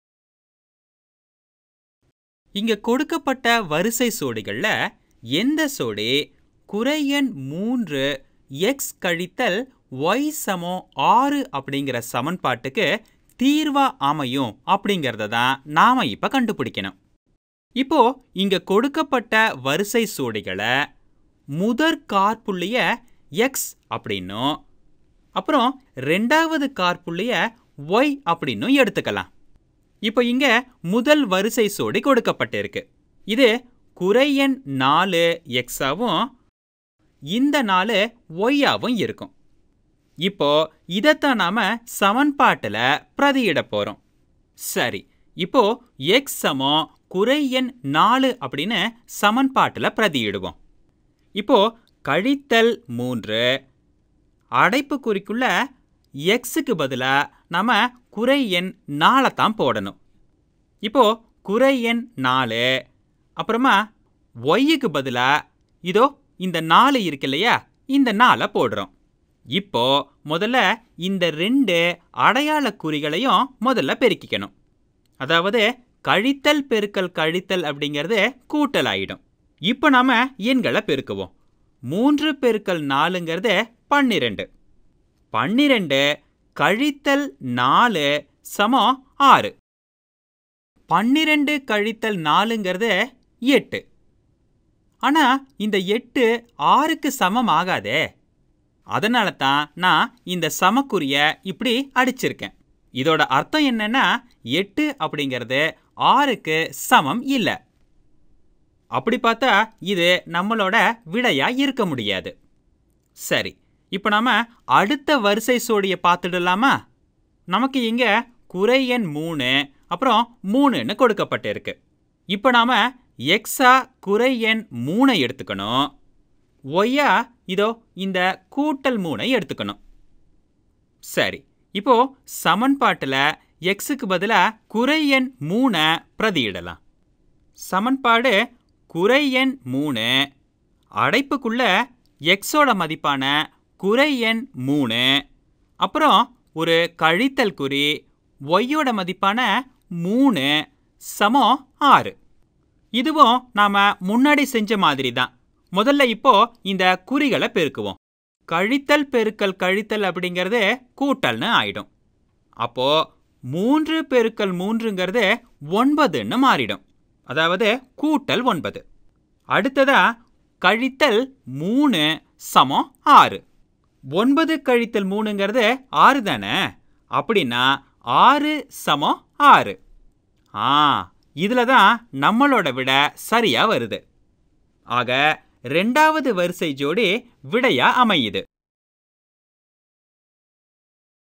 வரிசை ஜோடிகளில், எந்த ஜோடி கீழ்க்கண்ட சமன்பாட்டிற்கு சரியான தீர்வாகும்
This video is a Tamil translation of Khan Academy's "Checking ordered pair solutions to equations example 1" video